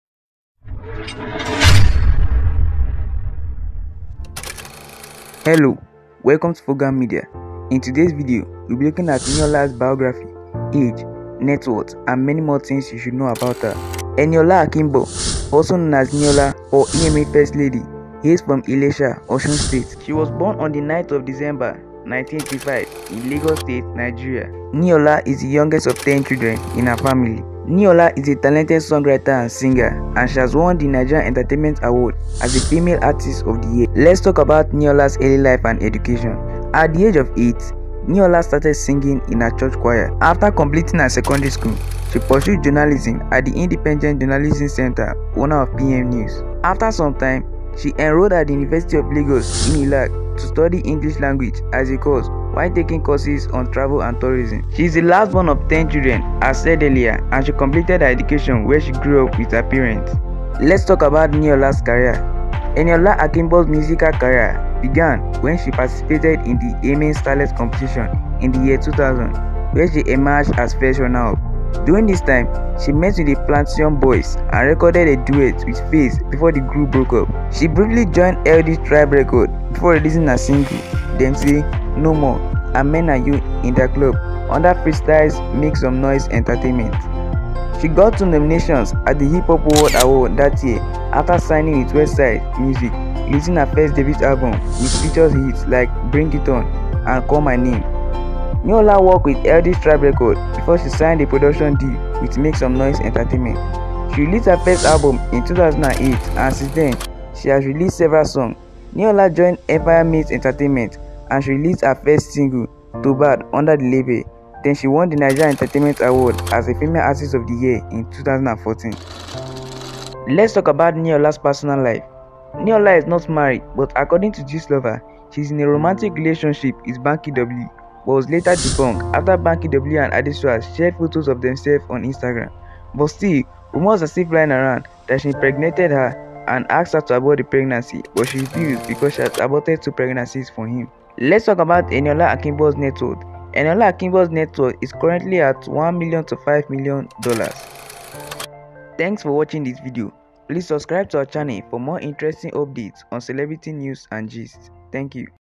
Niyola Biography, Age, Networth, Spouse, Marriage Life, And More
In this video, you will know about Niyola's biography, age , Networth Career and many more things you should know about her.

KINDLY JOIN THOUSANDS OF PEOPLE ON OUR TELEGRAM CHANNEL FOR TRENDING VIDEOS 💥💥